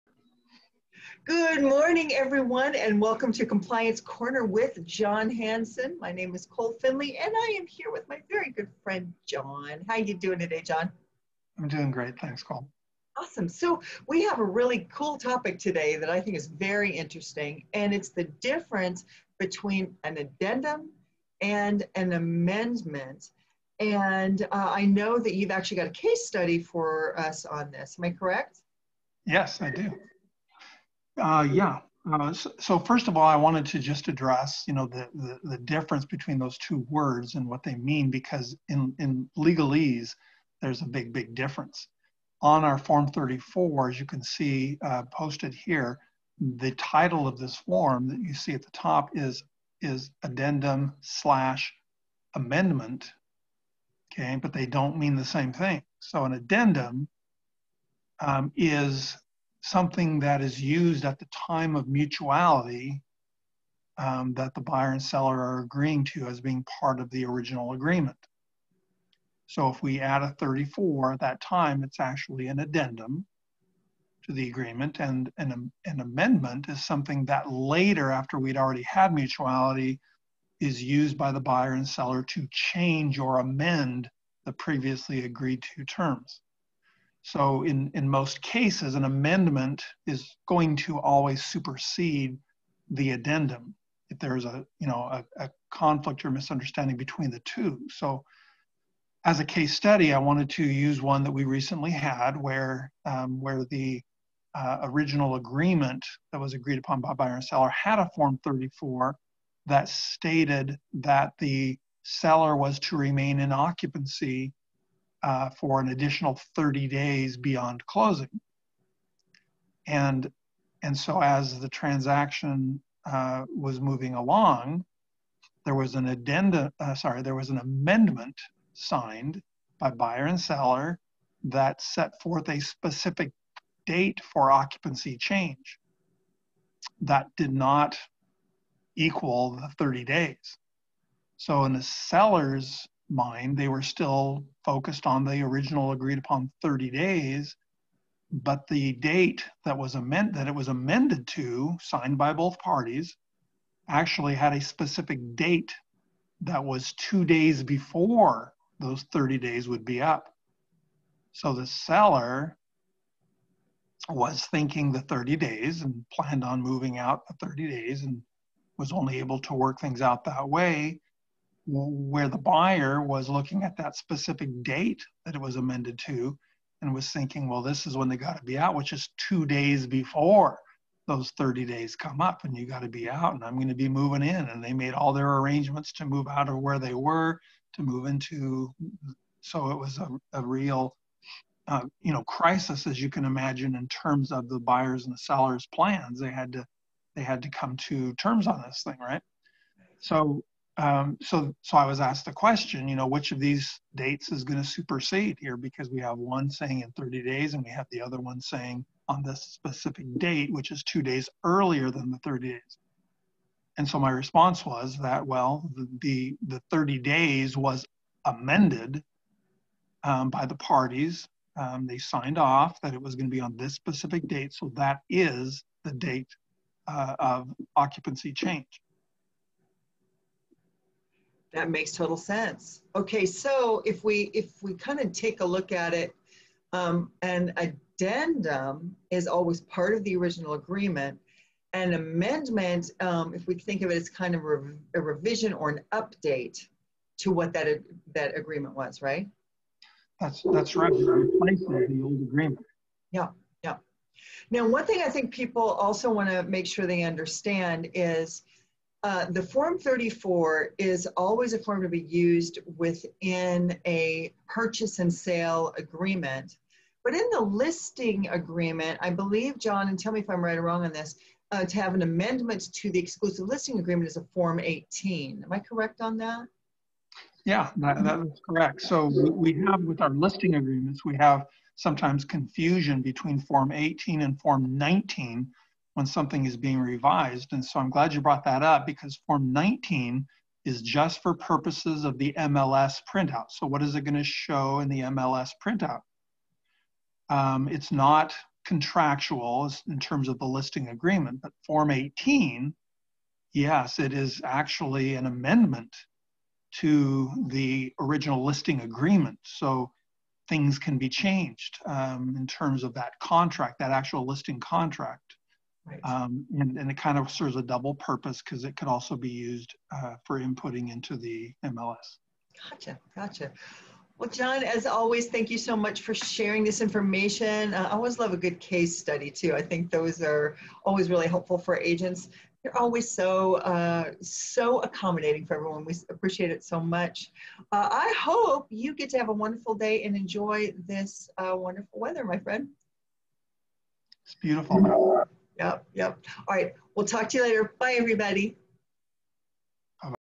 Compliance Corner with John Hanson - Is it an addendum or an amendment? Do You Know The Difference?
Join host Kole Finley and Designated Broker John Hanson as they discuss the difference between an addendum and an amendment in a purchase and sale agreement.  Recorded September 10, 2020.